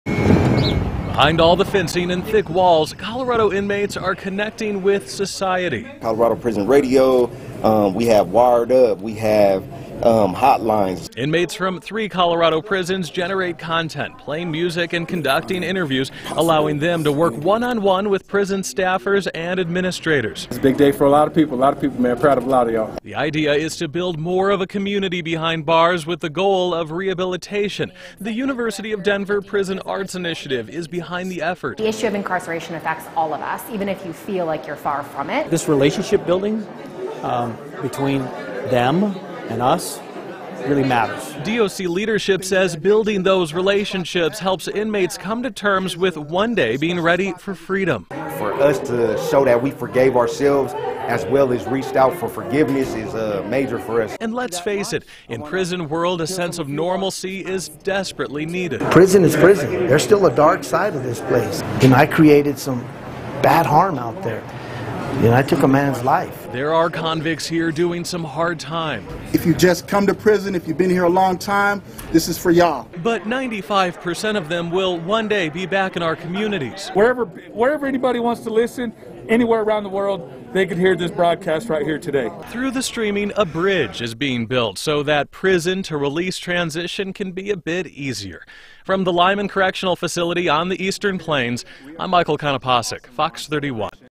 First-of-its-kind Colorado Prison Radio launches
A new streaming radio station allows listeners to go behind the walls of Colorado prisons. Michael Konopasek reports.